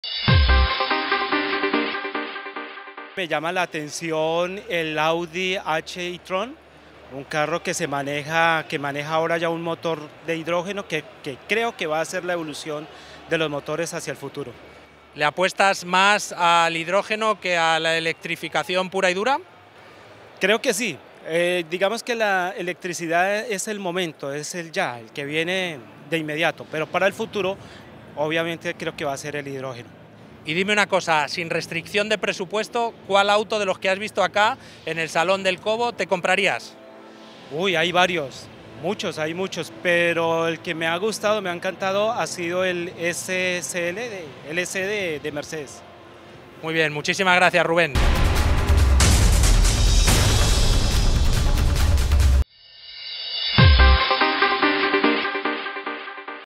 Autoshow de Detroit 2016. En la opinión de: Rubén Gonzalez/Revista Turbo Colombia
Rubén gonzález de la revista Turbo (Colombia), nos comparte sus modelos favoritos. También nos dice cuál auto se compraría.